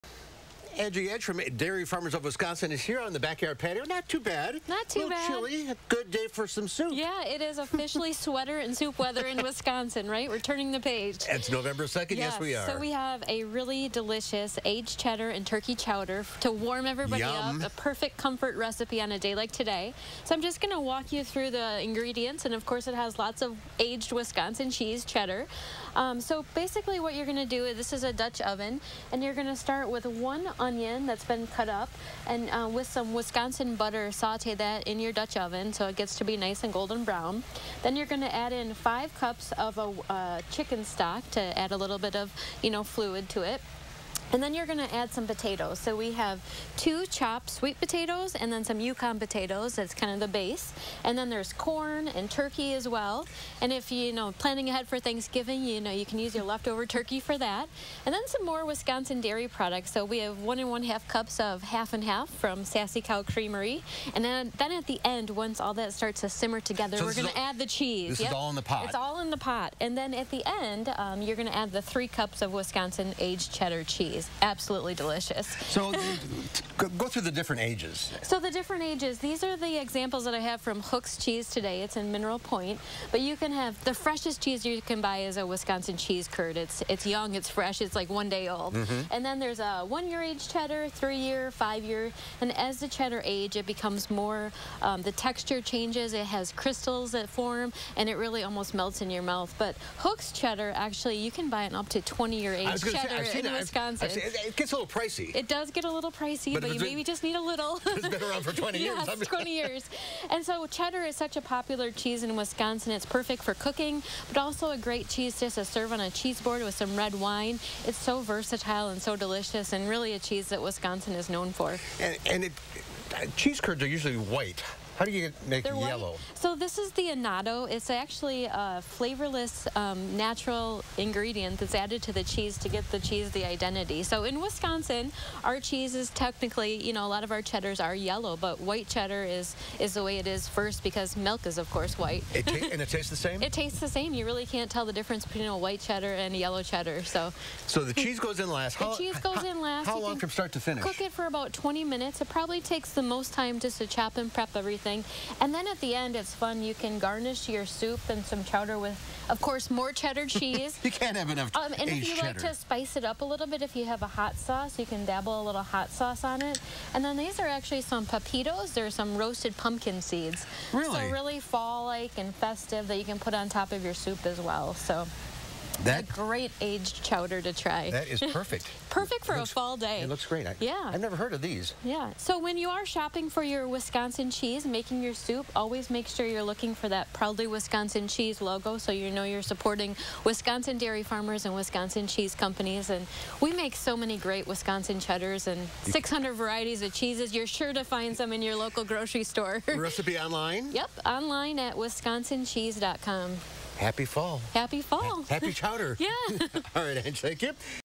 Angie Edge shares cozy chowder recipe perfect for fall
Angie Edge from the Dairy Farmers of Wisconsin shares a recipe for an aged cheddar turkey chowder.